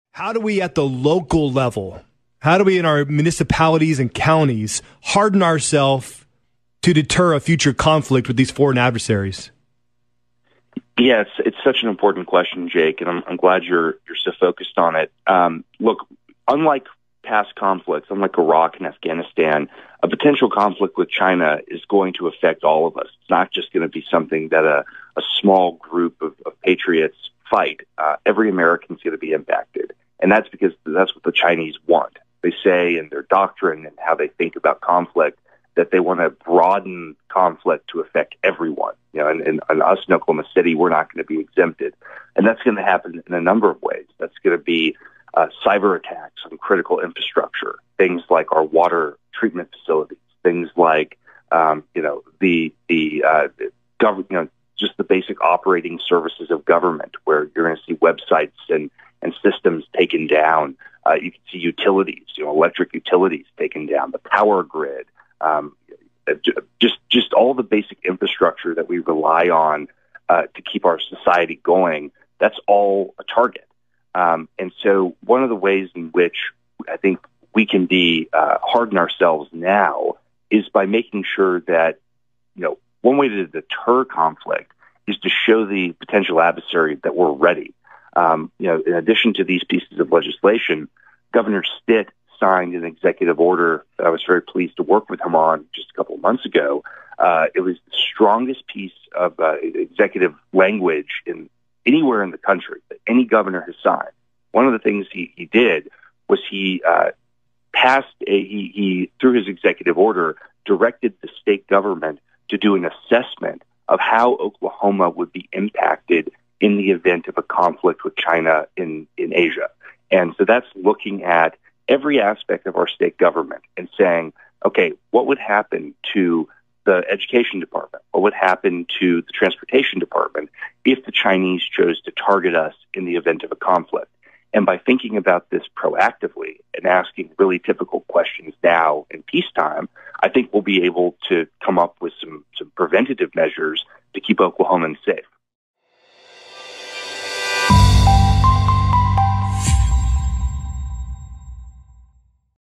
10/21/2024 美国前国家安全委员会办公厅主任亚历克斯·格雷讲述州政府和地方政府如何更好地应对中共带来的日益增长的威胁：“与中共国的潜在冲突将影响我们所有人。遏制冲突的方法之一就是向潜在对手表…
10/21/2024 Former NSC Chief of Staff Alex Gray outlines how state and local governments can better prepare themselves from growing threats posed by the CCP: “A potential conflict with China is going to affect all of us. One way to deter conflict is to show the potential adversary that we’re ready.”
10/21/2024 美国前国家安全委员会办公厅主任亚历克斯·格雷讲述州政府和地方政府如何更好地应对中共带来的日益增长的威胁：“与中共国的潜在冲突将影响我们所有人。遏制冲突的方法之一就是向潜在对手表明我们已经做好准备”。